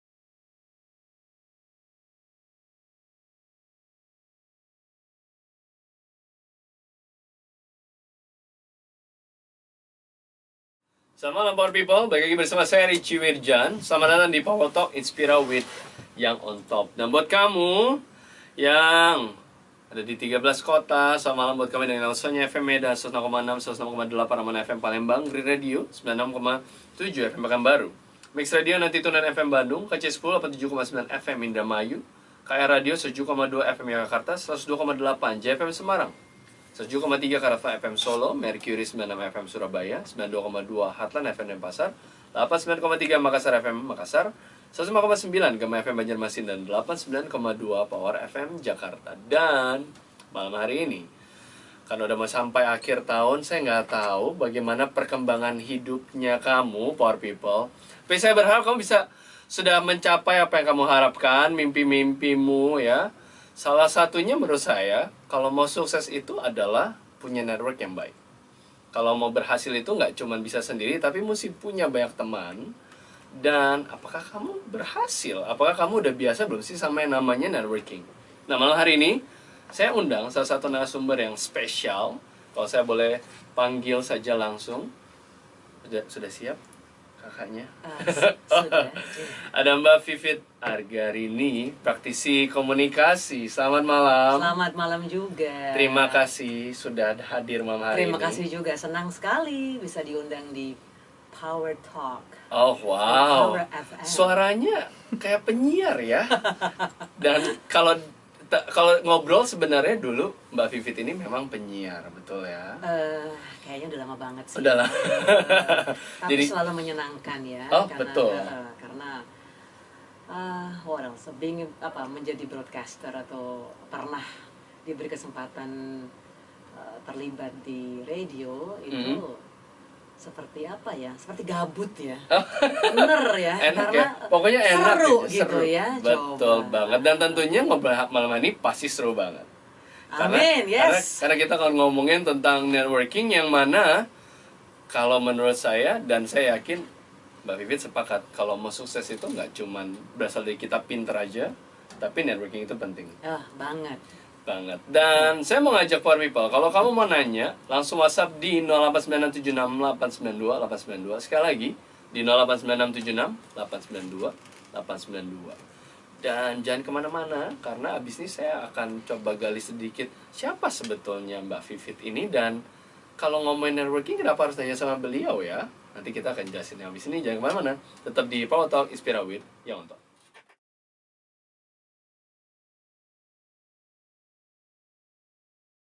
Vivid F. Argarini: Networking, Modal Sukses yang Gak Kalah Penting
Narasumber InspiraWithYOT: Vivid F. Argarini, Communication Practitioner